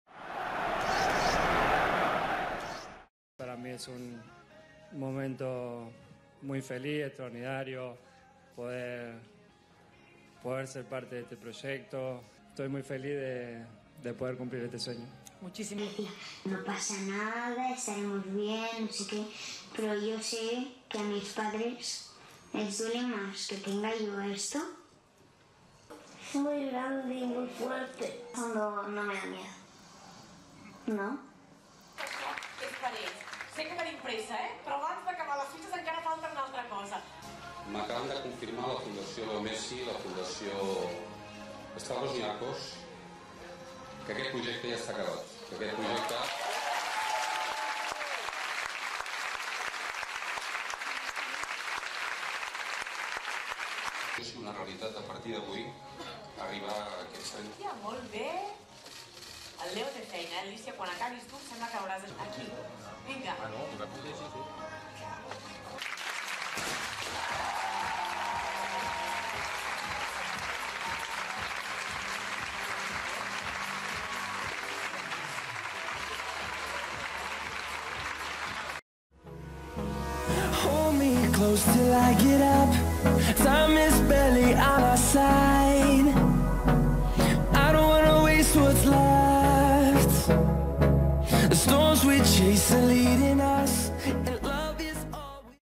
¡Grande! Messi se emociona en un acto contra el cáncer infantil ⚽ FC Barcelona ⚽ 2018
🔸 El futbolista argentino Lionel Messi mostró hoy su perfil más solidario con su apoyo a la lucha contra el cáncer infantil y asistió al inicio simbólico de la construcción del SJD Pediatric Center de Barcelona, el centro oncológico más grande de Europa con capacidad para atender a 400 niños pacientes."Estoy muy feliz por formar parte de este proyecto. Agradezco a toda la gente que ha hecho posible que sea una realidad. Ojalá podamos curar a todos los chicos y combatir una enfermedad tan cruel como el cáncer", declaró el jugador argentino del Barcelona. Messi se emocionó durante el acto.

La Fundación Messi y la Stavros Niarchos Fundation aportaron conjuntamente los casi tres millones de euros que faltaban para llegar a los 30 presupuestados para iniciar el proyecto.Además, ambas fundaciones se comprometieron a seguir sumando en el proyecto y a que puedan llegar los tratamientos a todas las partes del planeta a través de becas.

⚙ BANNER: @HypnotikGraphs